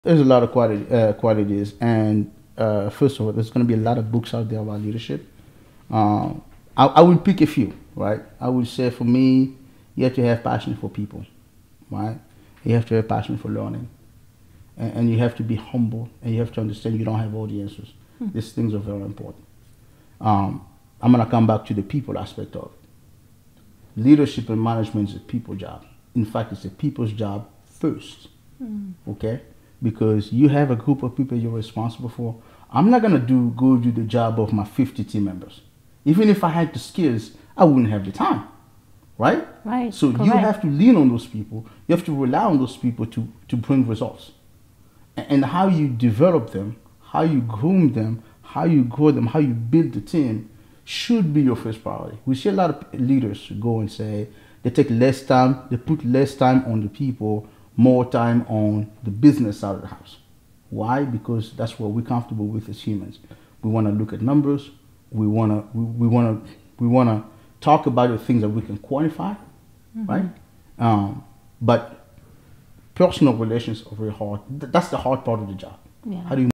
Unlocking the Secrets to Effective Leadership | Yves Laurent Sivuilu
Yves Laurent Sivuillu is a Senior Director at a prominent tech company in the world with success in building diverse teams who excel in technical expertise and delivery.
He has strong leadership and business skills derived from networking, computing, and cybersecurity.
His ability to align team members’ skills, customer needs, and business objectives to achieve significant results unmeasured.

Join us as Yves Sivuilu talks about leadership in the workplace and how he navigates the day to day challenges that he faces on and off the job!